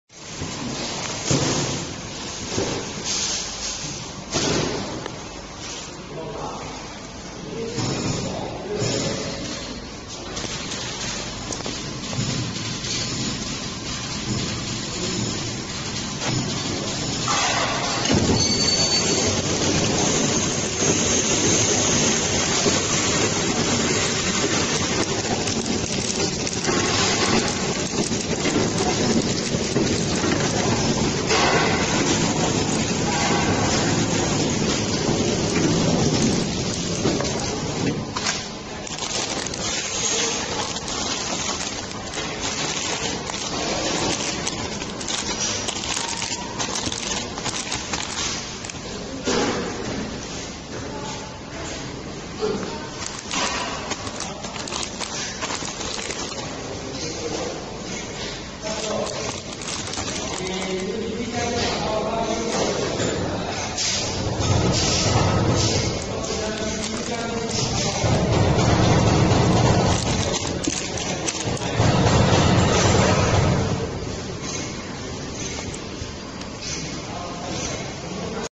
Down paper flow pack machine for 2pcs noodles with chain at knife
We are manufacturer of packing machine in Foshan,China.
Welcome to visit factory.